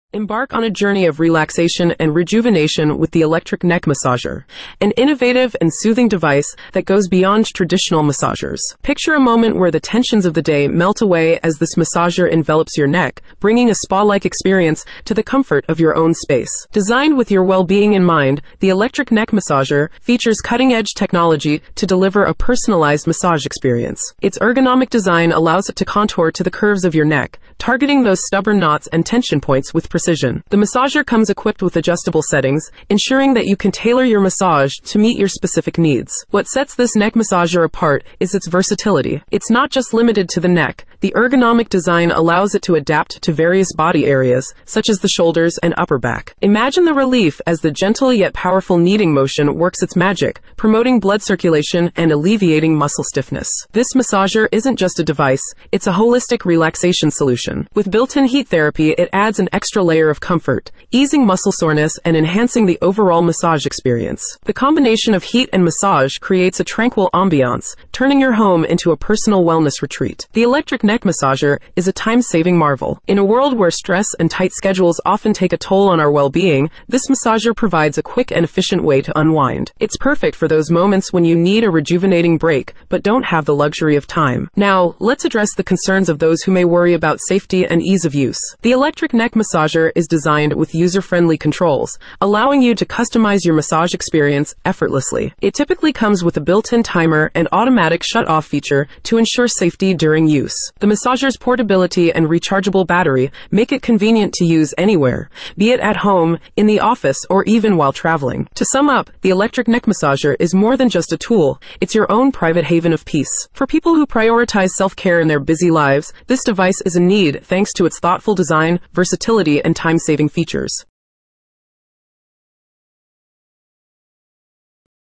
Relieve Tension & Stress Instantly Ultimate Neck Massager Review 2024
Looking for the best neck massager to soothe your stress away? Join us as we review the top-rated neck massagers of 2024! Whether you're dealing with neck pain, tension headaches, or just need to unwind after a long day, we've got you covered. Discover the features, benefits, and real user experiences with these innovative devices. Click to watch and find your perfect neck massager today!"

Feel free to adjust these suggestions to better fit your specific content and style preferences